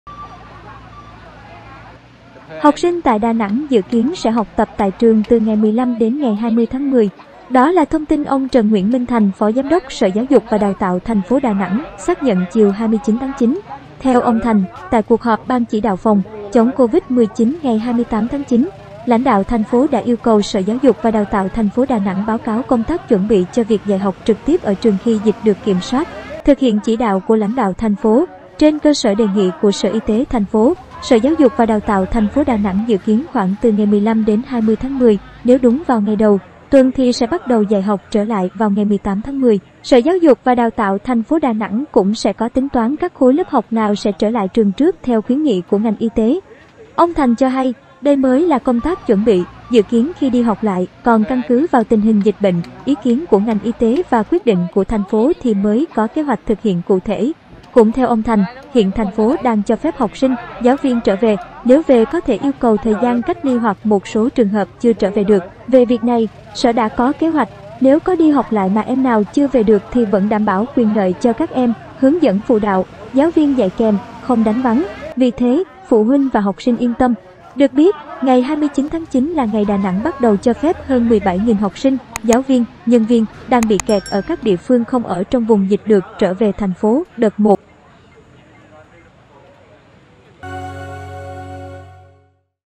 Học sinh tại Đà Nẵng dự kiến sẽ học tập tại trường từ 15 đến 20-10
Đó là thông tin ông Trần Nguyễn Minh Thành - phó giám đốc Sở Giáo dục và đào tạo TP Đà Nẵng - xác nhận chiều 29-9.